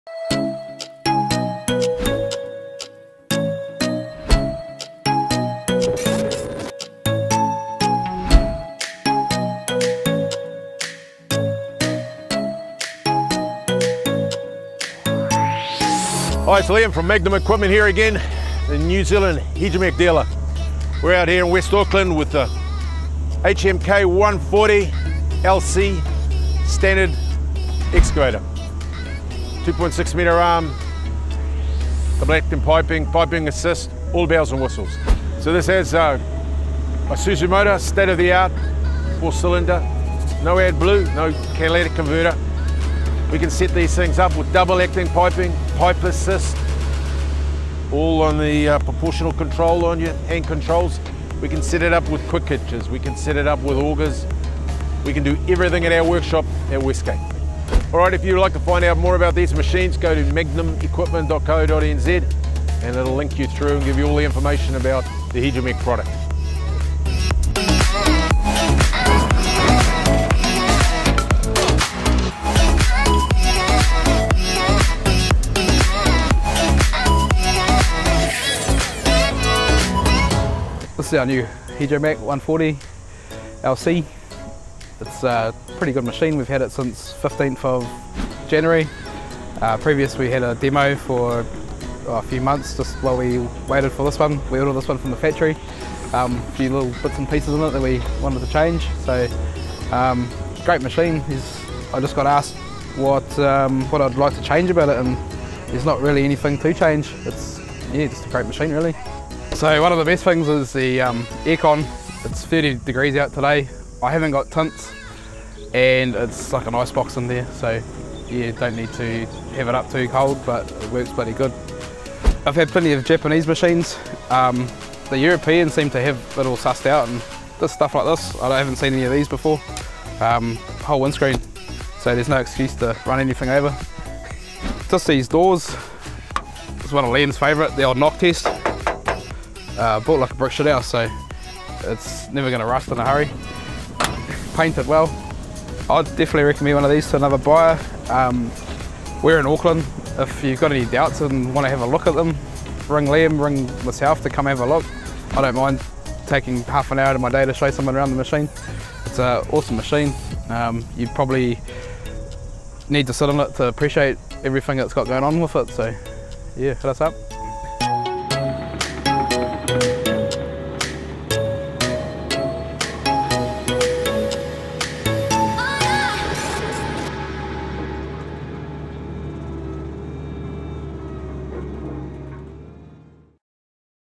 Hidromek HMK140LC Tested | Deals on Wheels
Deals on Wheels headed to West Auckland to check out the HİDROMEK HMK140LC in action. WBL Civil Landscapes, who have purchased one of the new Hidromeks, share their thoughts.